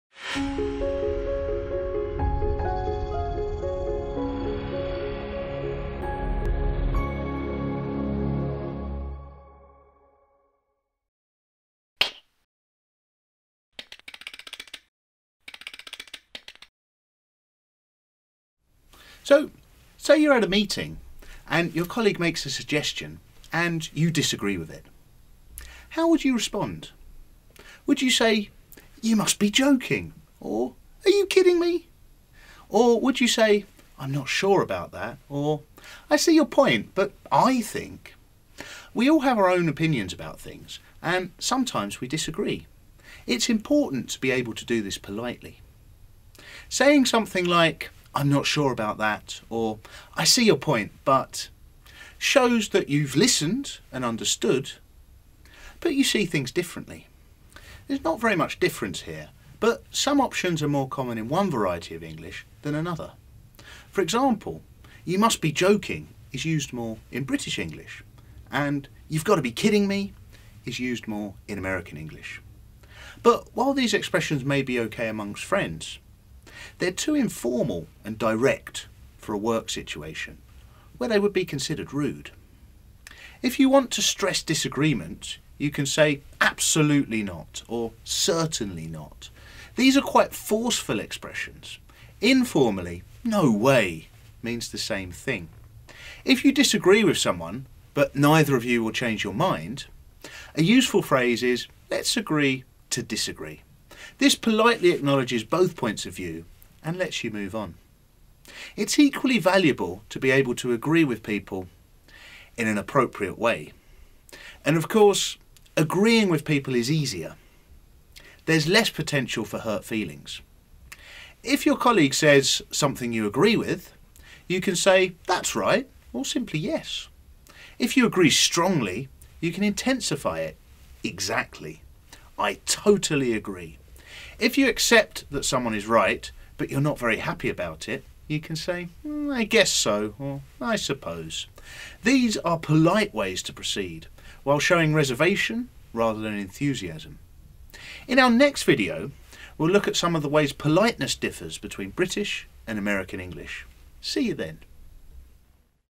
Real World English: agreement & disagreement
In this episode of our series, Ed Pegg talks about appropriate ways of agreeing or disagreeing with friends and colleagues.